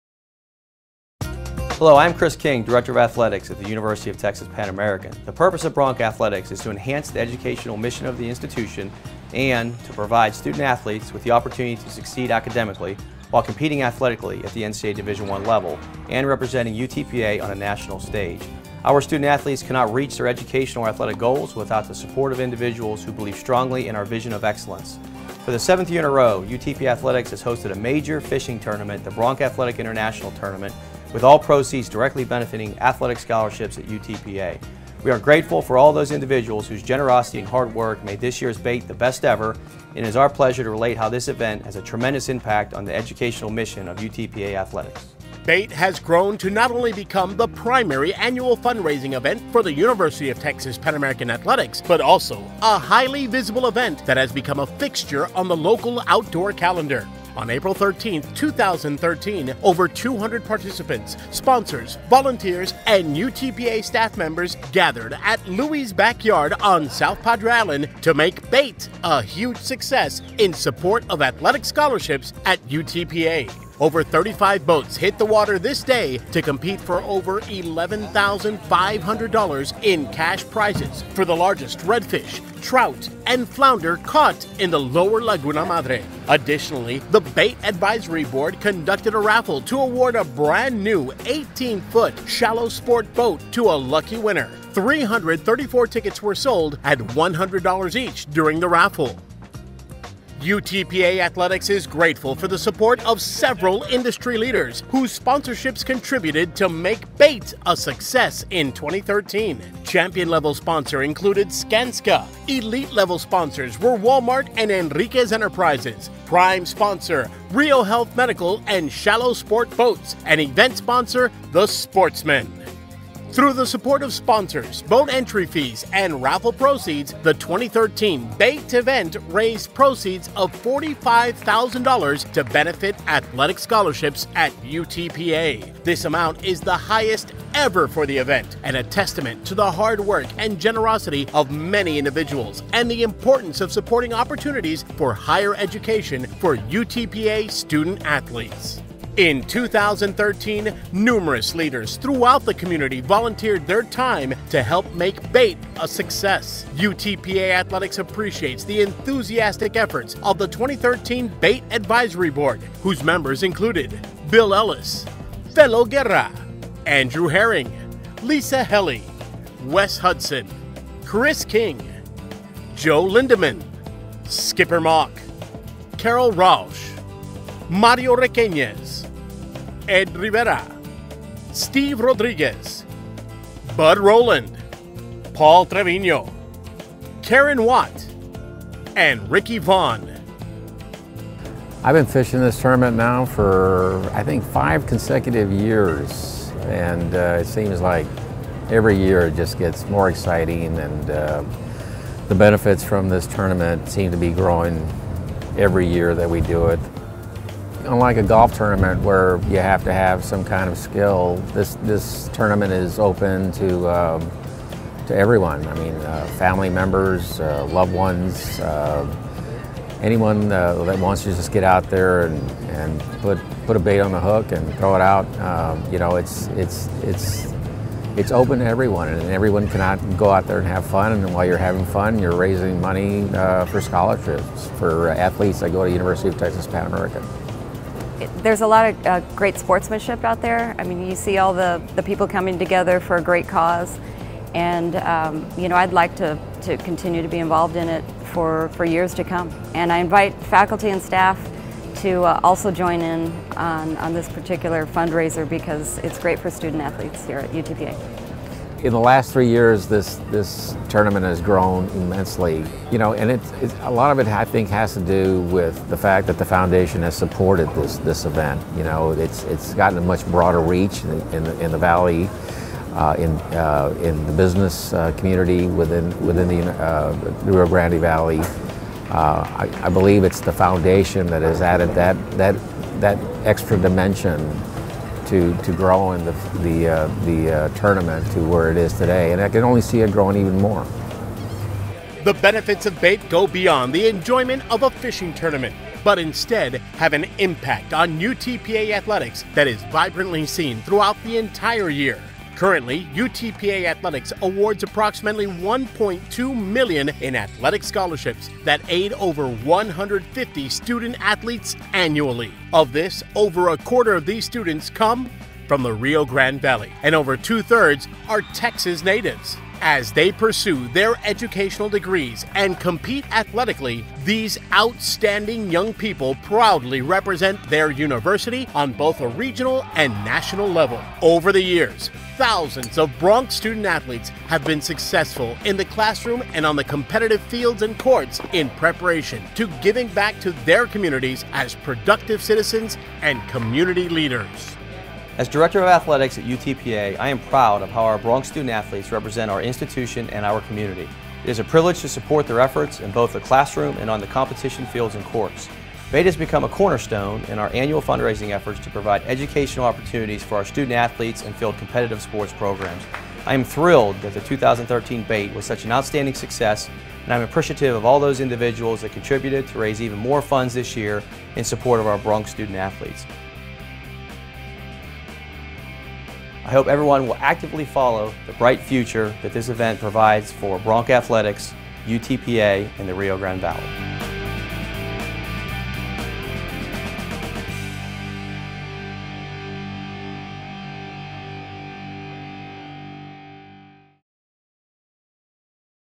Registration Now Open for Eighth Annual B.A.I.T.
Dec. 9, 2013 -- SOUTH PADRE ISLAND, Texas -- The University of Texas-Pan American Department of Intercollegiate Athletics announced on Monday that registration is now open for the eighth annual Bronc Athletic International Tournament (B.A.I.T.), a bay fishing tournament to raise money for student-athlete scholarships, which will take place on Saturday, April 12.

Teams of up to four can register for $300 per boat until April 4. Late registration, which costs $400 per boat, runs April 5 through the end of the captains' dinner on April 11 at 6 p.m. at Louie's Backyard.

Registration can be completed online at UTPABroncs.com/BAIT, by calling UTPA Athletics at , or in-person at the UTPA Athletics office. The final late registrants will be allowed to sign-up at the captains' dinner.

The top eight teams will share $10,000 in prize money. The winning team will receive $4,000.

Anglers will compete to catch redfish, trout and flounder. The winner will be decided based on the total weight of all eligible fish caught. There is also a Big Fish Pot for the largest of each of the three species of fish caught.

Participants will have most of Saturday to fish, with weigh-in at 4 p.m. at Louie's Backyard. In order to participate in the weigh-in, teams must have either a skipjack or a one-eyed jack card, which can be purchased at the captains' dinner.

Additionally, a limited number of raffle tickets for a brand-new, 18-foot Shallow Sport boat, valued at over $35,000, are available. The sale of raffle tickets will be capped at 500. Raffle tickets cost $100 each and can be purchased by contacting Karen Watt at or . Those holding raffle tickets do not need to be present at B.A.I.T. to win.

Every raffle ticket is good to free admission to one 2014 UTPA baseball game after April 12.

UTPA Athletics spends over $1.2 million annually on scholarships that benefit over 175 student-athletes. About one-third of these student-athletes hail from the Rio Grande Valley and more than 60 percent are from the state of Texas.